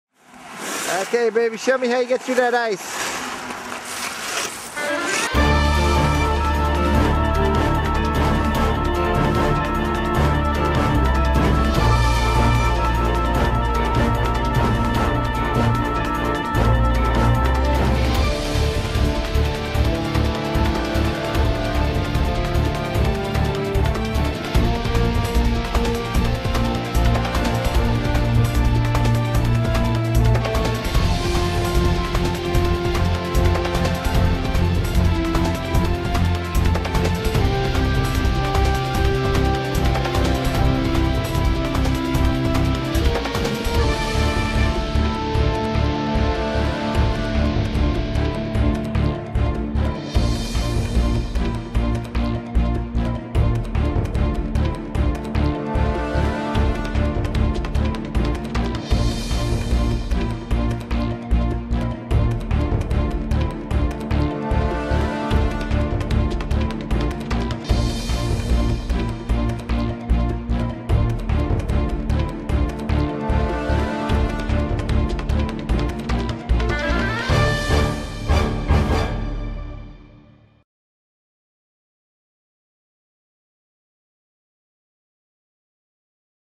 The Gutster Ice & Snow
Never send a shovel in to do a Gutster's job.  See how the Gutster handles even the toughest ice with ease.